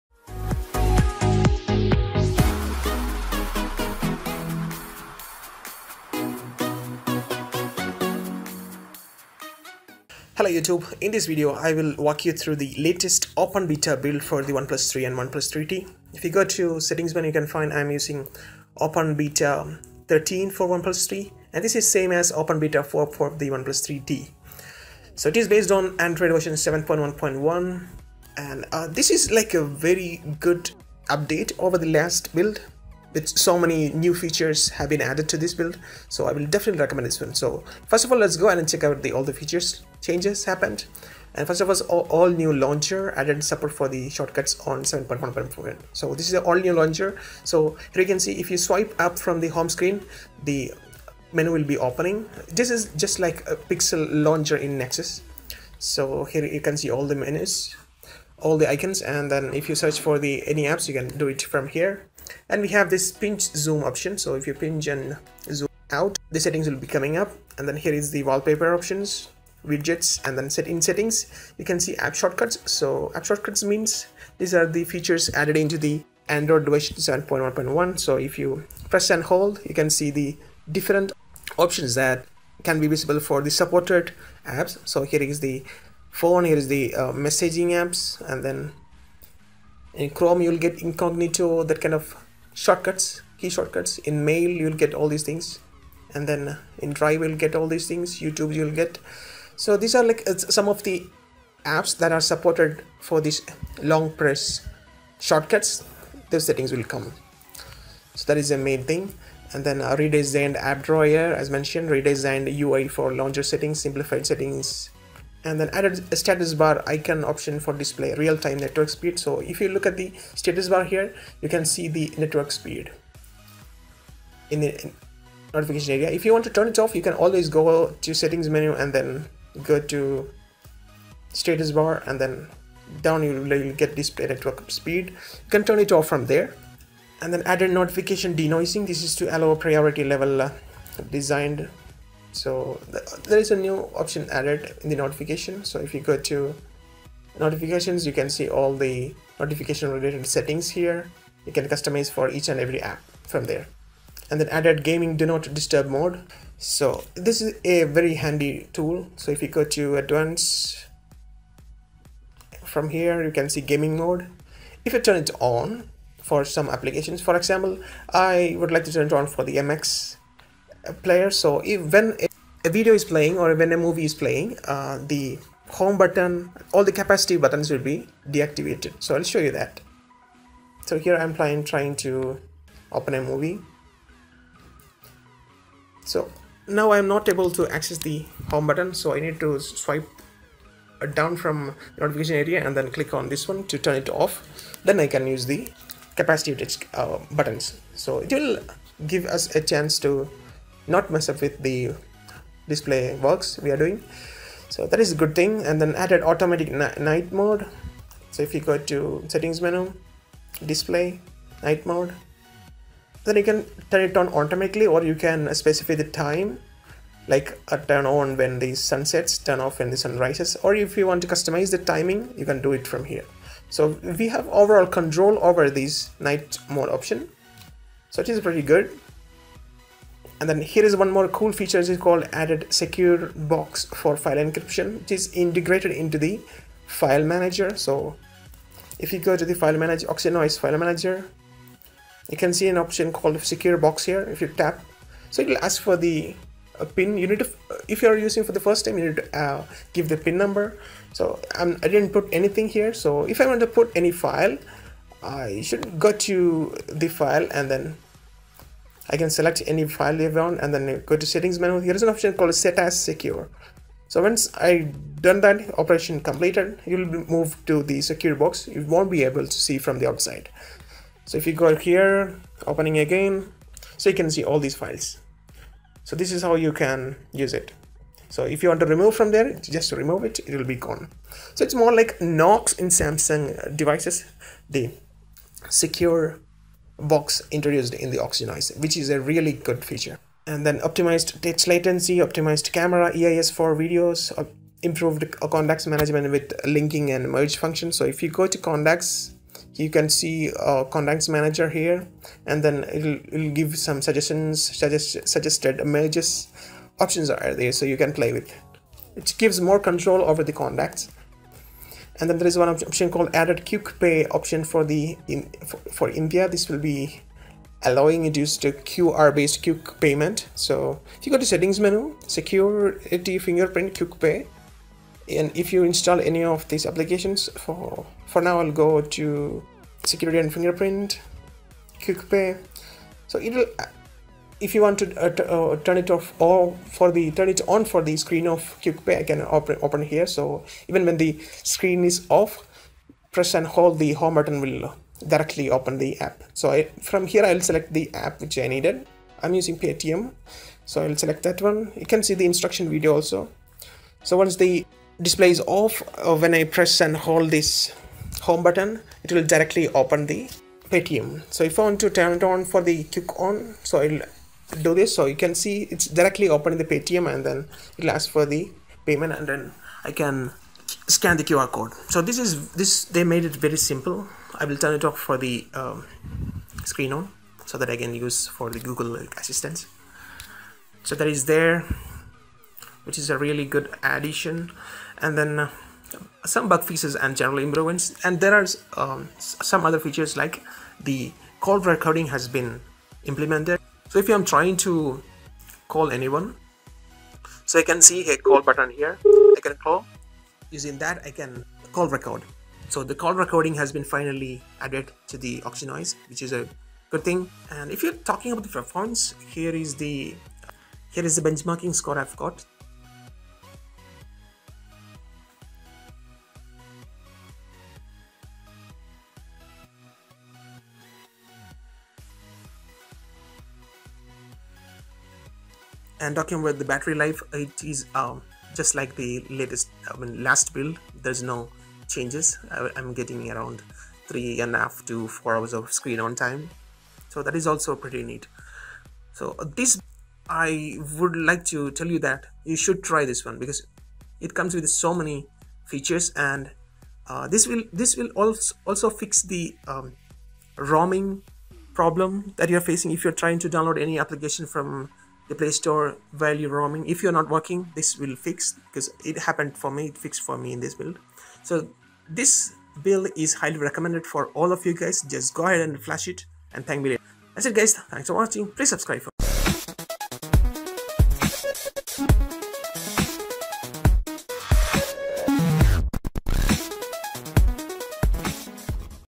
Why You Should Update Your OnePlus 3/3T to OxygenOS Beta 13/4 - Awesome Features
This video will show you Why You Should Update Your OnePlus 3/3T to OxygenOS Beta 13/4.

Music Credits...